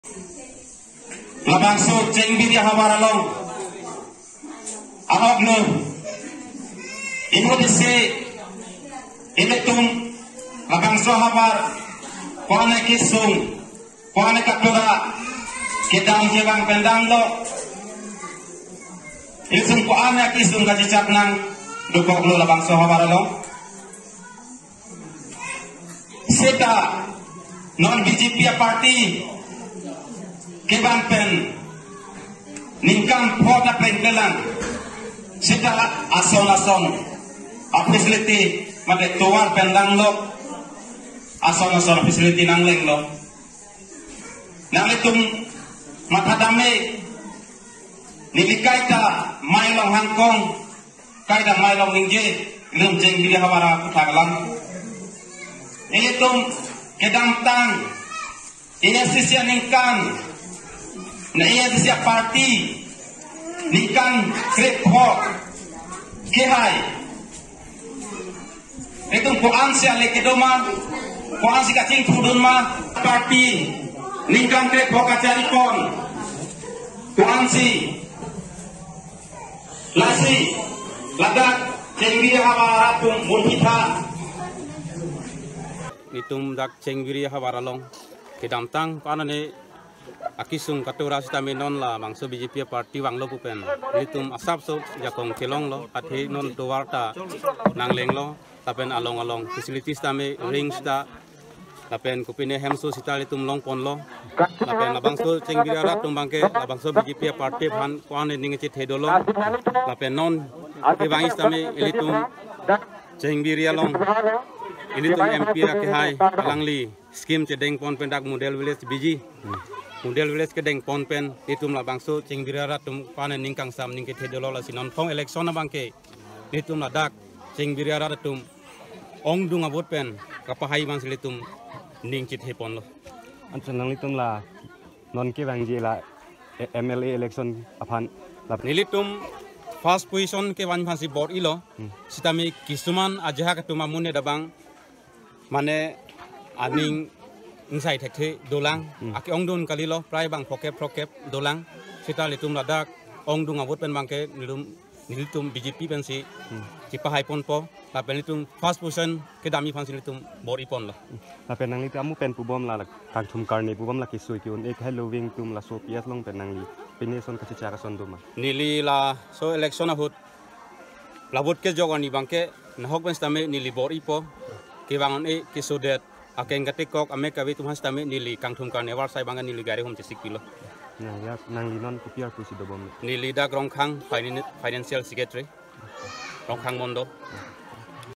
Rongkhang BJP workers holds campaign for candidate Rupsing Teron
Correspondent
Dongkamukam, 23 Mar'2021: The BJP Rongkhang Mondol Committee held a campaign programme for candidate Rupsing Teron 20-Baithalangso (ST) LAC at Phosong, Chengbiri PS under Rongkhang Constituency in West  Karbi Anglong district on March 22.

Senot Enghee, one of the speaker at the beginning of the campaign spoke to the persons present in the programme to vote for Rupsing Teron and help him win the MLA election.

He mentioned that in the past times local people have faced many difficulties like transport and communications in the Chengbiri area but after the BJP has come into power, in less than five years they have been facilitated with roads connectivity and many other services.

Speaking to the medias, he stated that ever since BJP came into power the locals have recieved developments like roads, ring-well, benefitted with PMAY schemes etc. and therefore Chengbiri residents have high hope to the ruling government in coming days too, he added.

Further, he mentioned that MP Horensing Bey assured to form a Model Village at Chengbiri PS while in return the locals will vote for BJP with high numbers.

Later, in the day of election, he will arrange transportation facilities to the sick and needy persons so that they can cast their respective vote, S. Enghee concluded his speech in the said meeting.